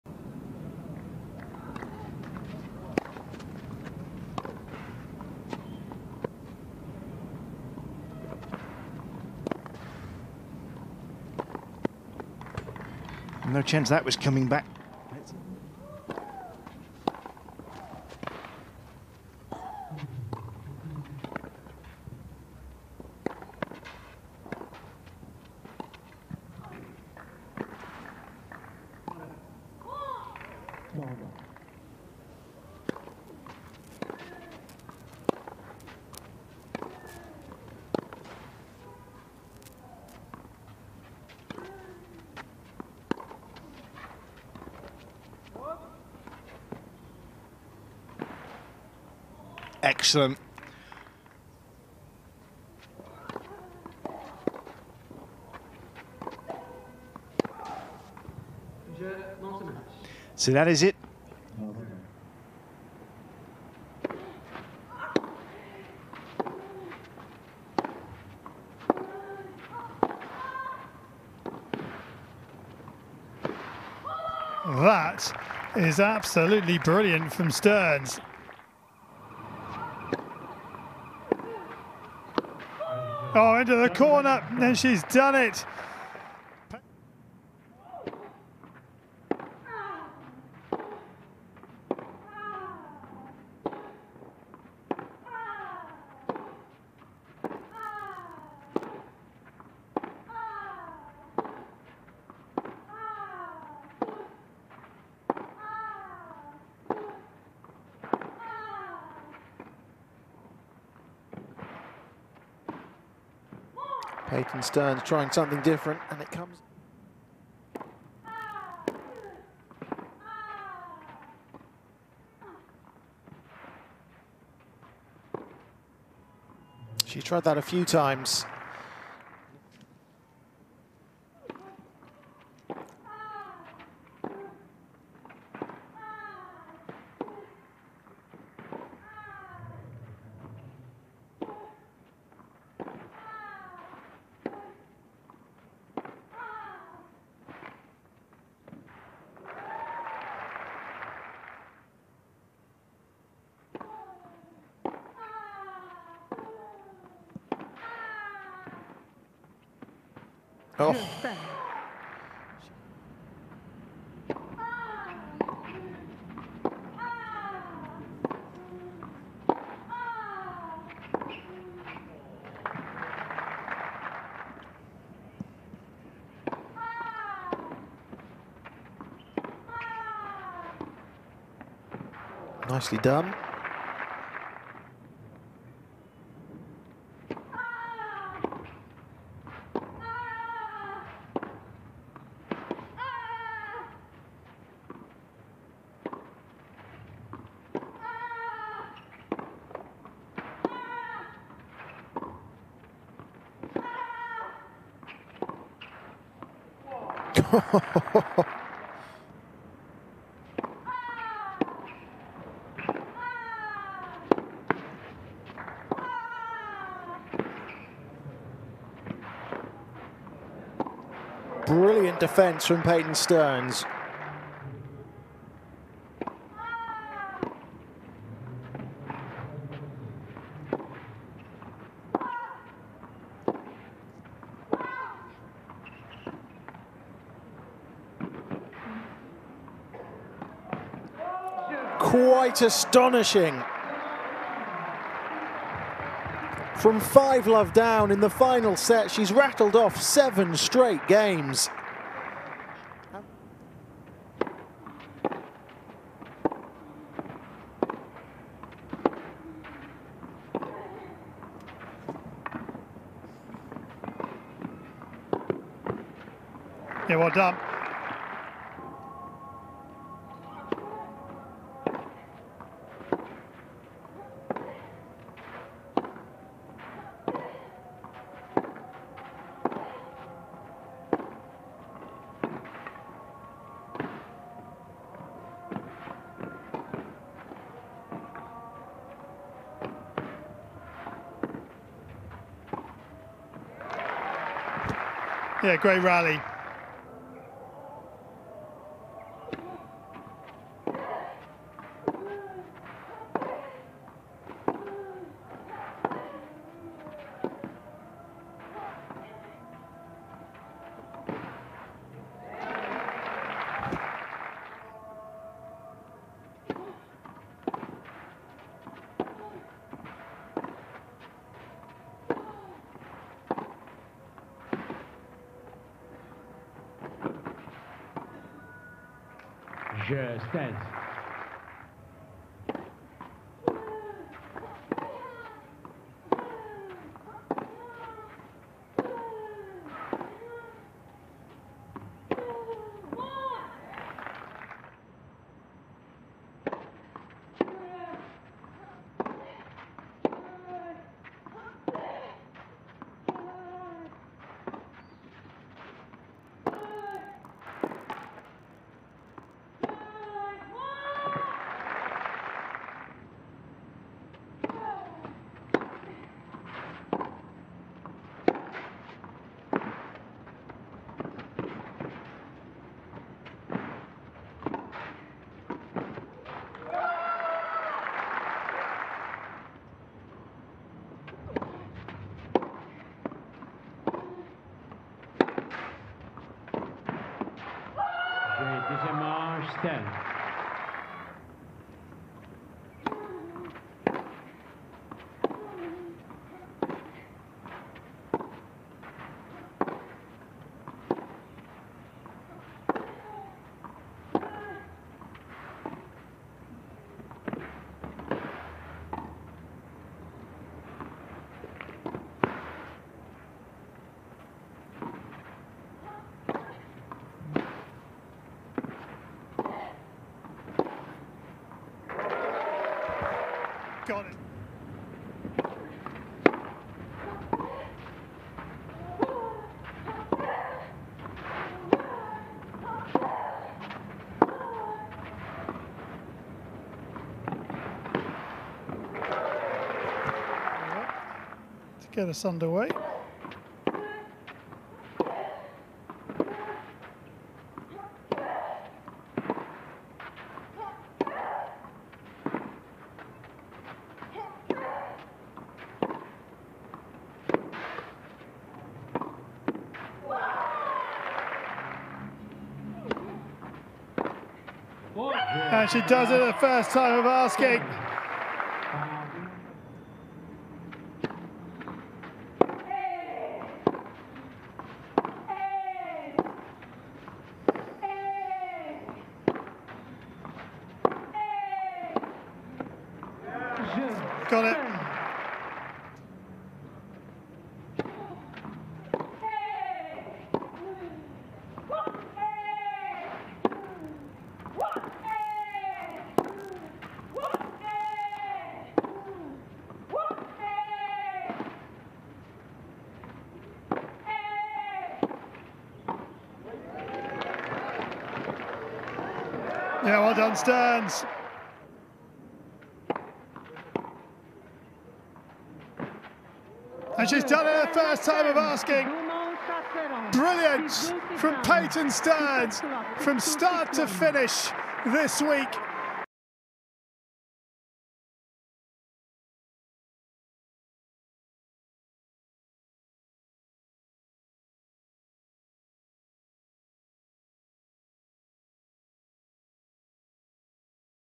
Peyton Stearns captures her first WTA singles title 🏆
Peyton Stearns had to survive a couple marathon matches to make this week's Grand Prix Son Altesse Royale La Princesse Lalla Meryem final, but it was a very different story on Saturday, where she eased to her first WTA singles title.

The 22-year-old American Stearns, ranked World No.81, defeated 66th-ranked Sherif to capture her maiden trophy on tour.

Watch Peyton's top plays from Rabat!